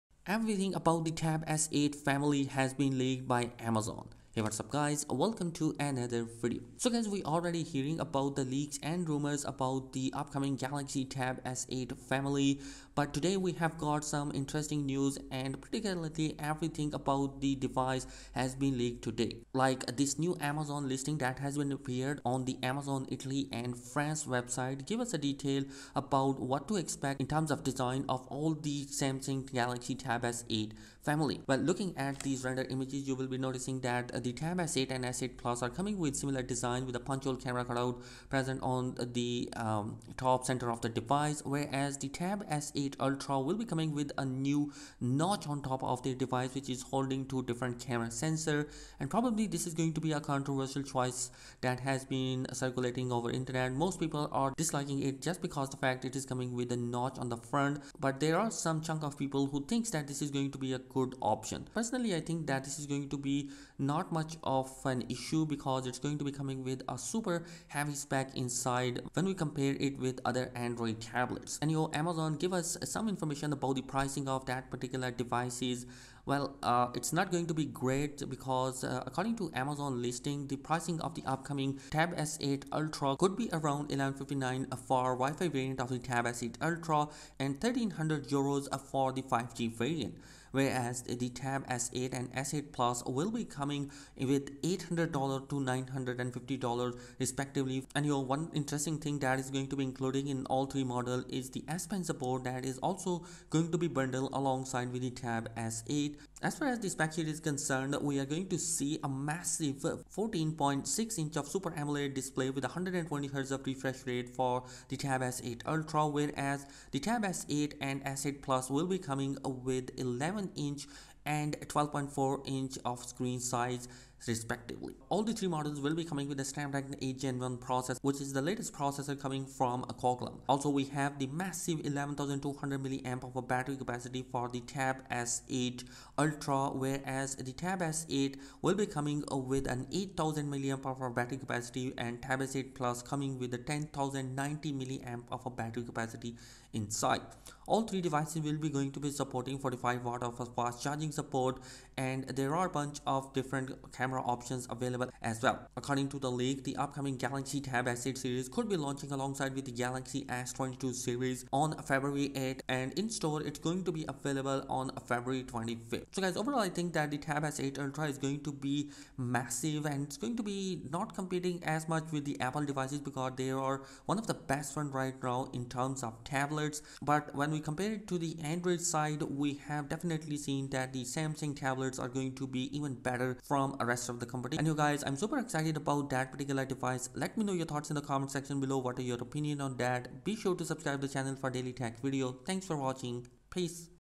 Samsung Galaxy TAB S8 Ultra - IT'S ALL HERE!!
Samsung Galaxy Tab S8 Ultra's official image is leaked by Samsung accidentally on the US support page website. Yes, Samsung's image of the Samsung Galaxy Tab S8 Ultra was accidentally shared on its own website. The image was discovered on a Bixby-related help page (by 91Mobiles). The notch on the tablet's display is readily visible in this image. A keyboard is attached to the tablet and should be sold separately. Surprisingly, the page is still up, and the image has not been removed (as of now). That's all we can deduce from the image, but we're hoping to discover more about the tablet in the future.